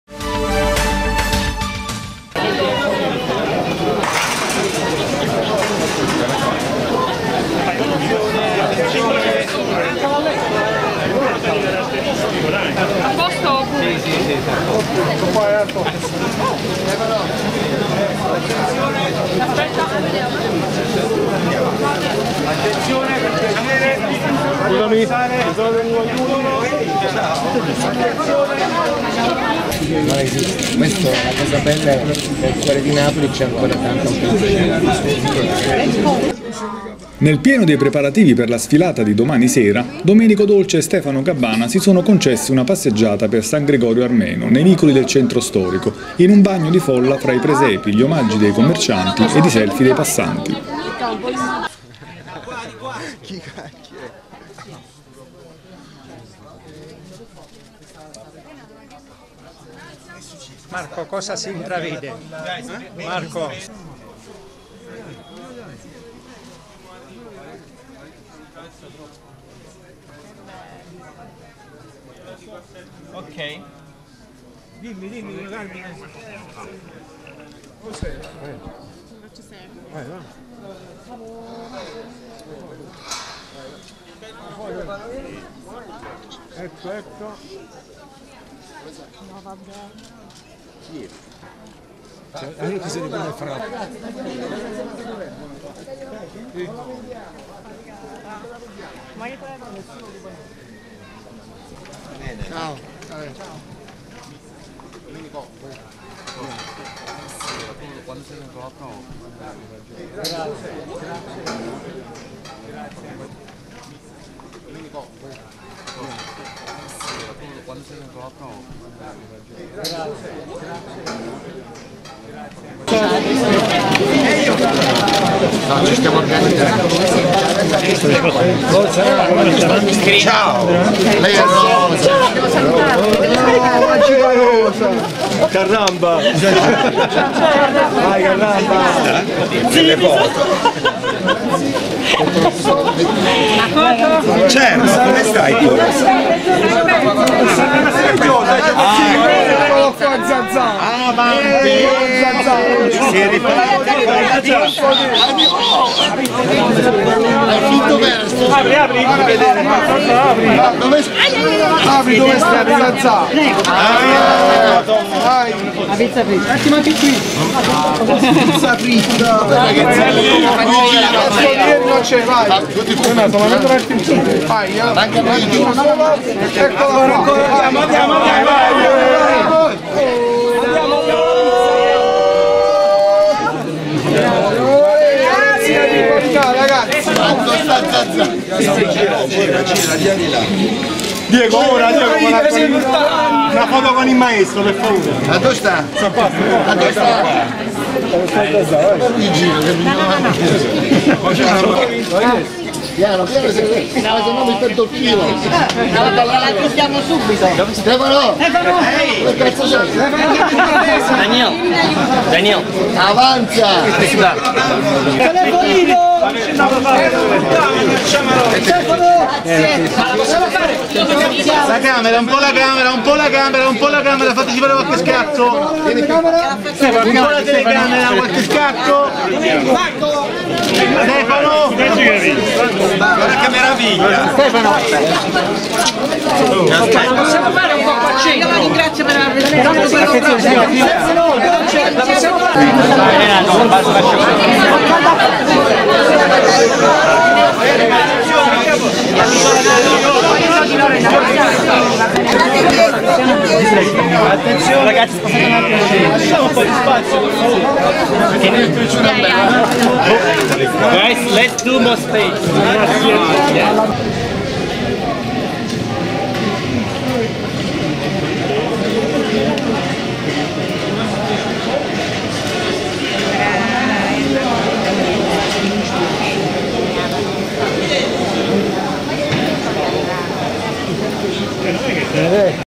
Dolce&Gabbana "sfilano" a San Gregorio Armeno
Nel pieno dei preparativi per la sfilata di domani sera, Domenico Dolce e Stefano Gabbana si sono concessi una passeggiata per San Gregorio Armeno, nei vicoli del centro storico, in un bagno di folla tra i presepi, omaggi dei commercianti e selfie dei passanti.